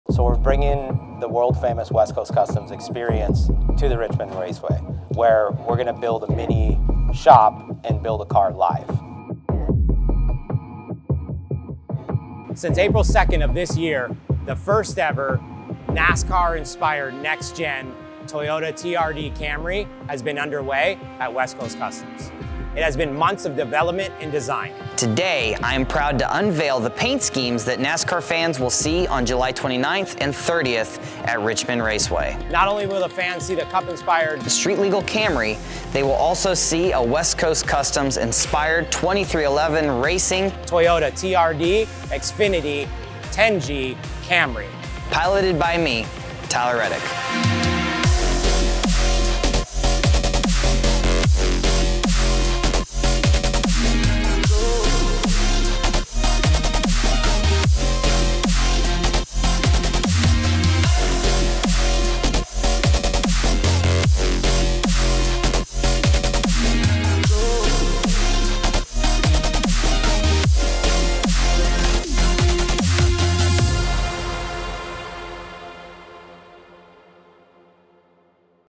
Paint scheme reveal | West Coast Customs street legal NASCAR build
Ryan Friedlinghaus, Tyler Reddick and  @WestCoastCustomsTV  unveil the paint scheme for the 2023 street legal NASCAR Next Gen car for the street.

Play every weekend!